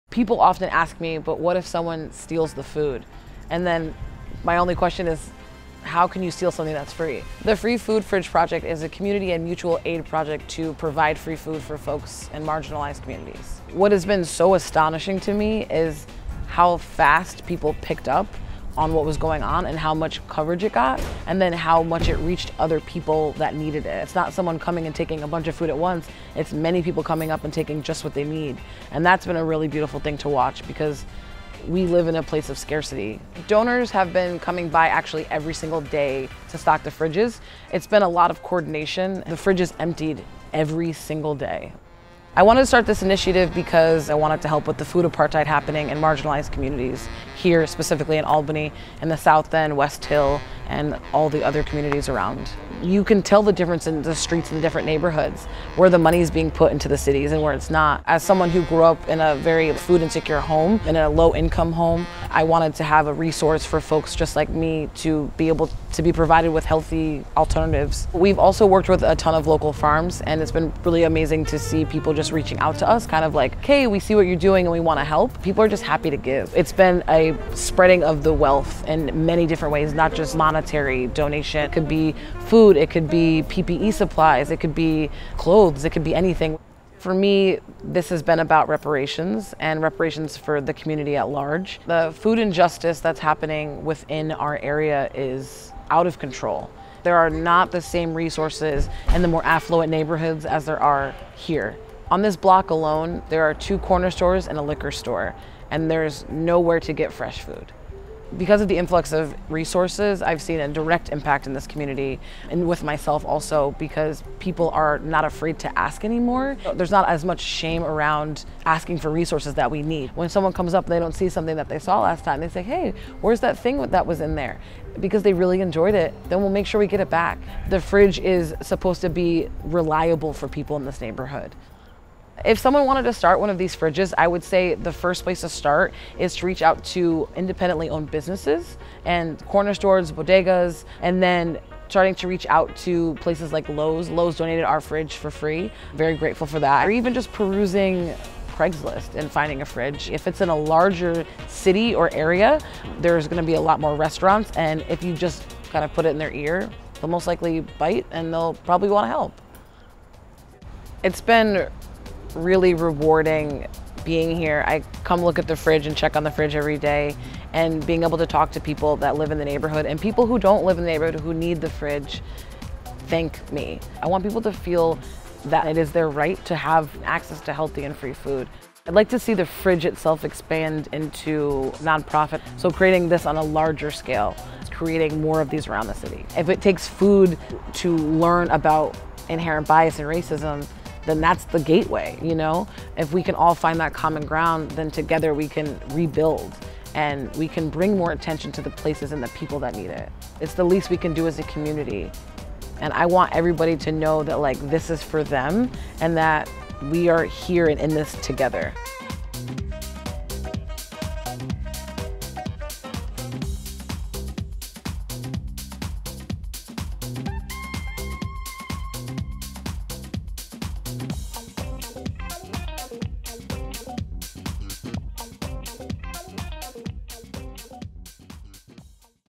This Free Food Fridge Should Be Everywhere
See how this community fridge provides free food to food insecure neighborhoods.

Goodful
Goodful

MUSIC

EXTERNAL CREDITS
Aiko Igasaki